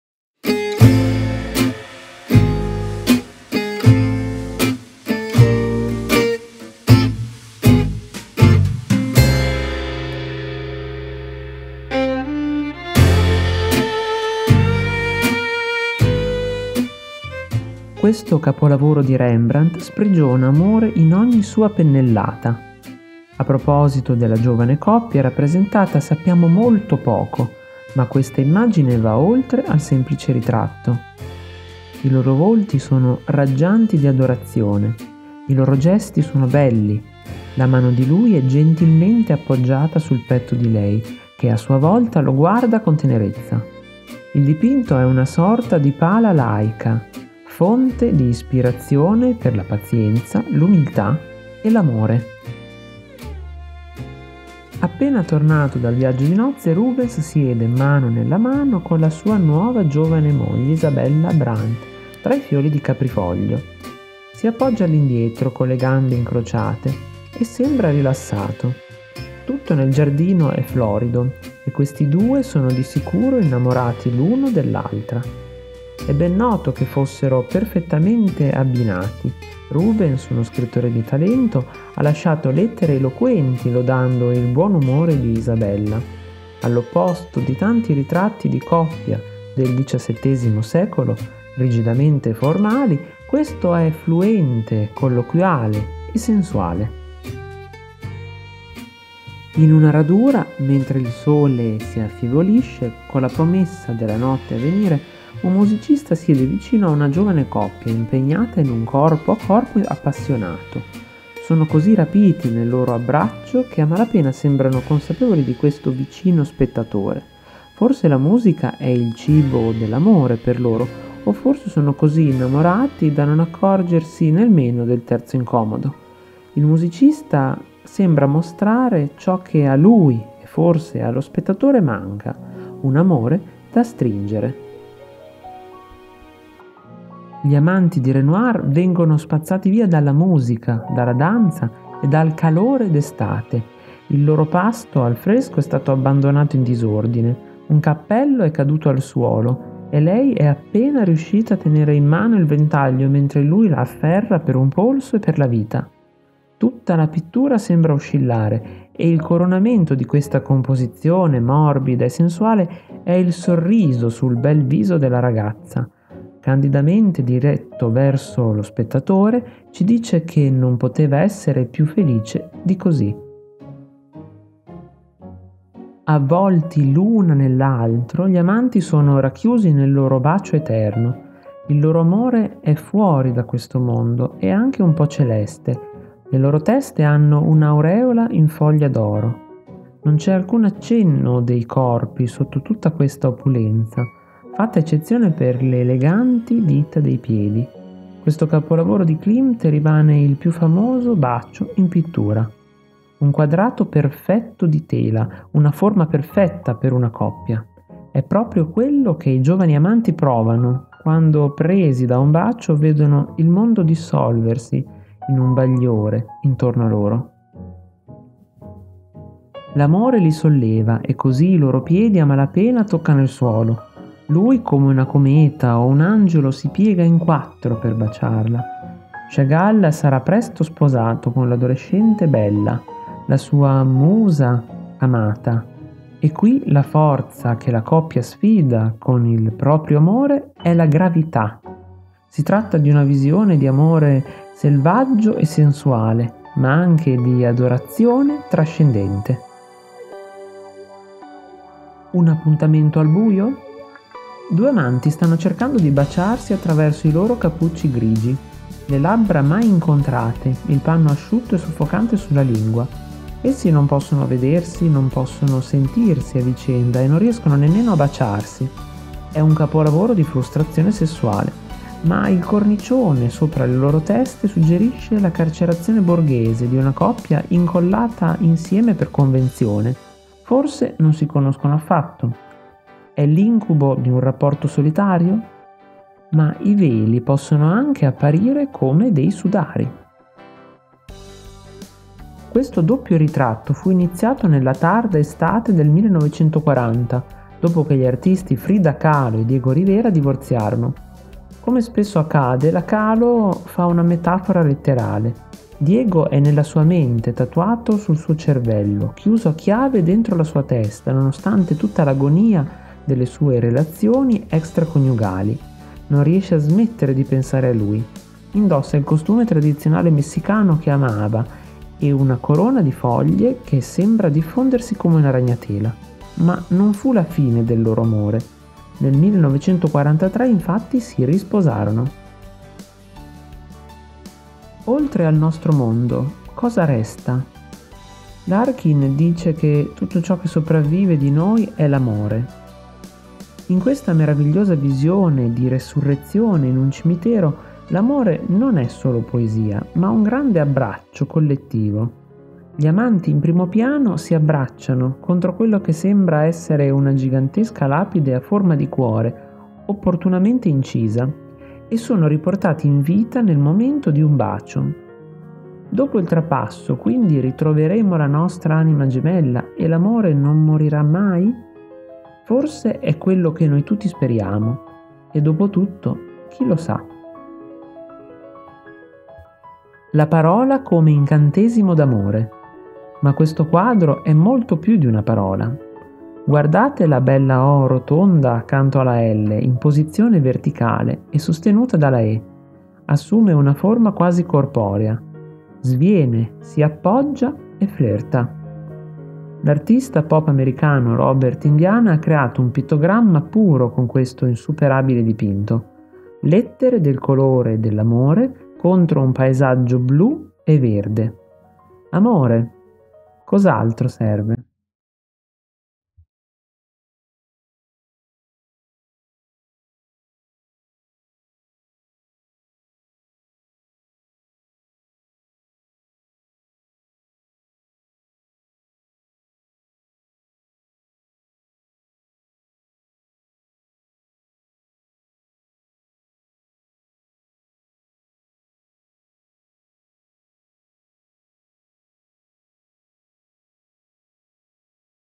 10 momenti d'Amore
10 opere d'arte per restituirvi sensazioni, immagini e suggestioni legate all'amore.
I testi sono tradotti da theguardian.com
▼▼CONTINUA SOTTO▼▼

Amore ... una parola che racchiude tantissime storie, raccontate anche in arte. Amori finiti tragicamente, lieti fini, amori spezzati e amori eterni. Amori segreti o non corrisposti. L'amore di una musa ad esempio spesso ha alimentato la creatività degli artisti.

Troverete un commento alle opere:
➡ Rembrandt, la sposa ebrea, 1666
➡ Pieter Paul Rubens, autoritratto con la moglie Isabella Brandt, 1609
➡ Antoine Watteau, la sorpresa, 1718
➡ Pierre-Auguste Renoir, ballo di campagna, 1883
➡ Gustav Klimt, il bacio, 1908
➡ Marc Chagall, il compleanno, 1915
➡ René Magritte, gli amanti, 1928
➡ Frida Kahlo, autoritratto come Tehuana, 1943
➡ Stanley Spencer, la resurrezione. Reunion, 1945
➡ Robert Indiana, love, 1965

Il video è sottotitolato in italiano, inglese, francese e spagnolo. Puoi contribuire anche tu a migliorare i sottotitoli in lingua straniera, accedendo nell’apposito spazio del canale Youtube.